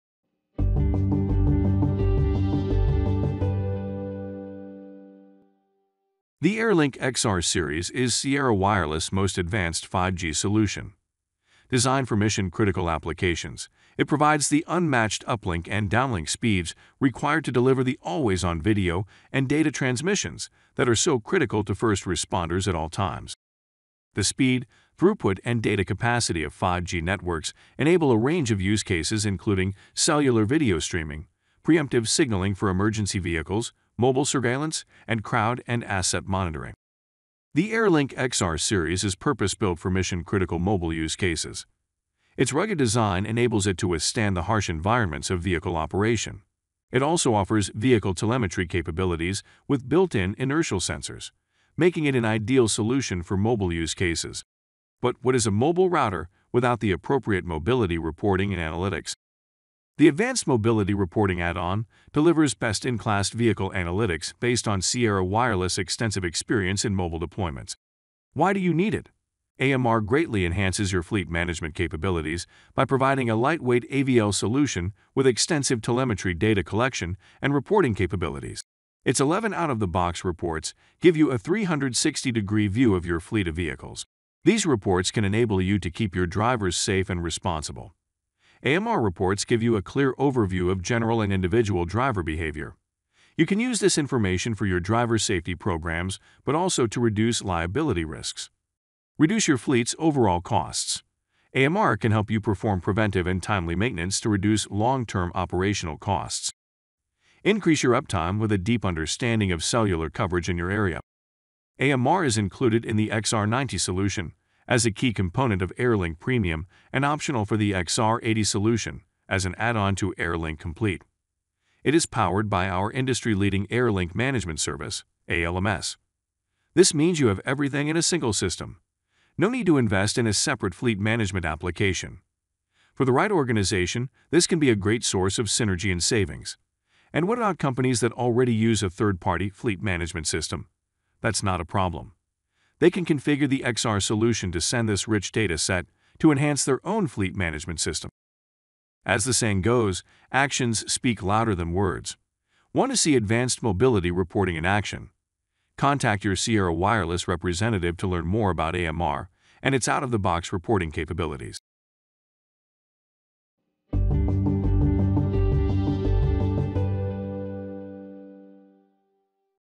Keep your drivers safe and reduce your fleet’s overall costs with Advanced Mobility Reporting
Purpose-built for mission critical mobile use cases, the XR Series Solution delivers extensive telemetry, data collection and reporting capabilities.  In this 3-minute video, see how Advanced Mobility Reporting (AMR) delivers best in class vehicle analytics that enable organizations to have a 360-degree view of their fleet of vehicles.